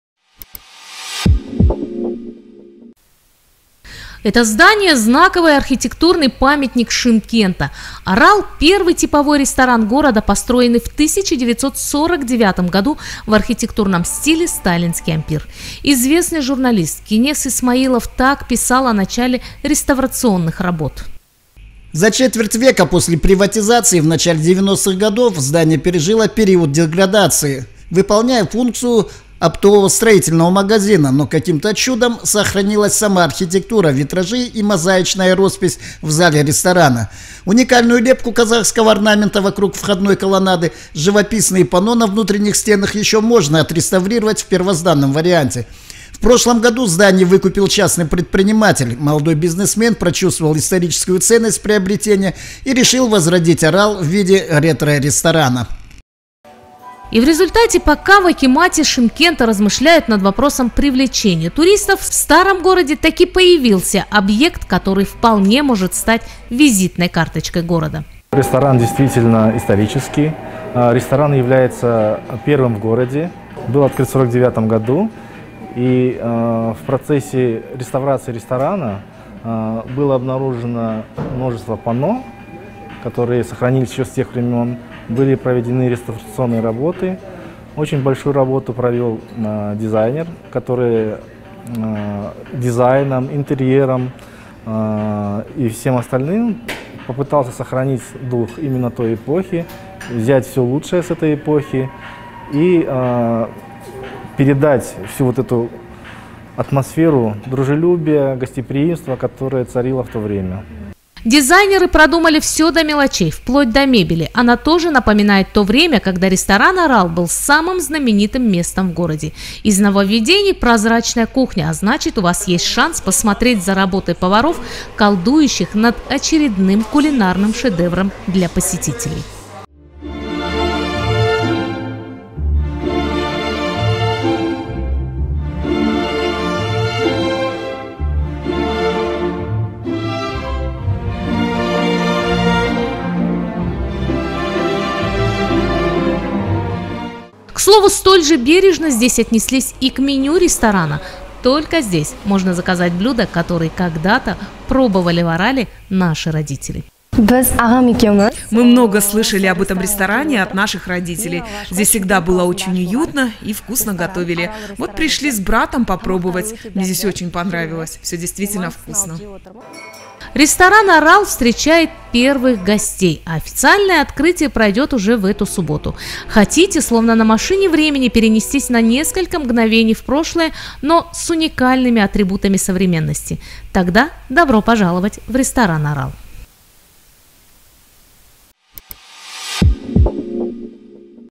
Легендарный ресторан "Арал" восстановили в Шымкенте
Легендарный ресторан «Арал», который вполне может претендовать на звание визитной карточки города, открыт в Шымкенте после реставрации. Новые владельцы к объекту, построенному 70 лет назад, отнеслись настолько бережно, что сохранили и воссоздали в интерьере дух того времени.